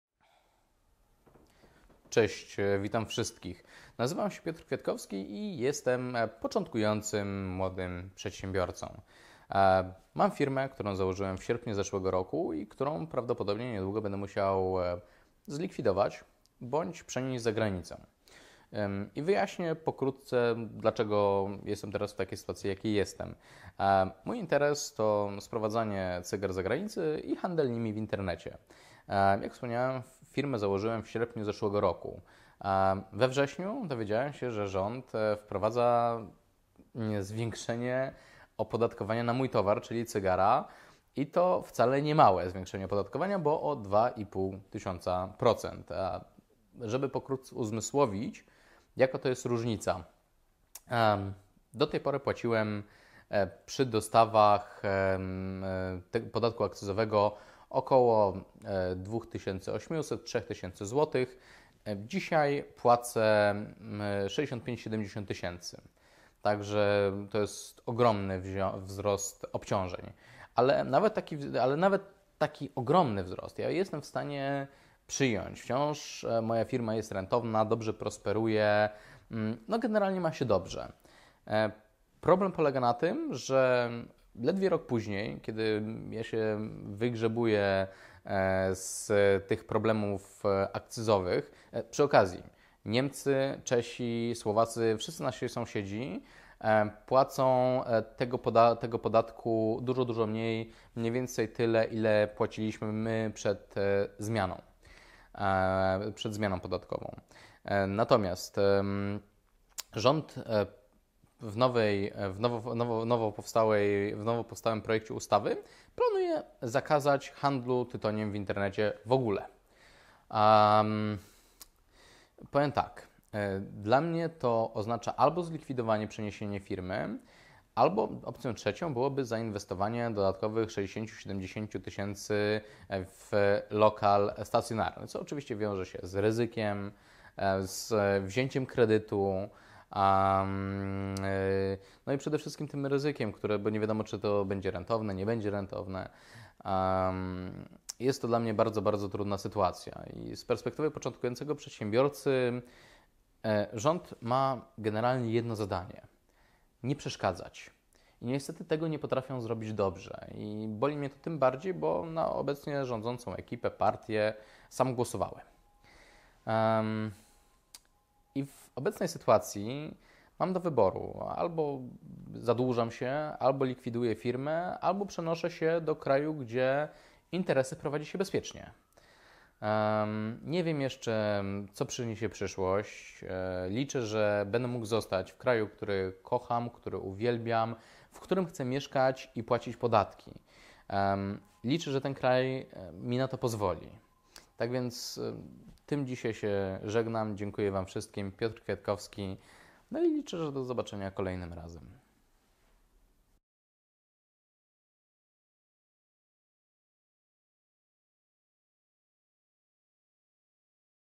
Biznes w Polsce, czyli dlaczego firmy przenoszą działalność
Dziś będzie mało cygarowo, za to sporo o smutnej rzeczywistości. Powodem nakręcenia tego filmiku jest zapowiedź delegalizacji handlu tytoniem w internecie.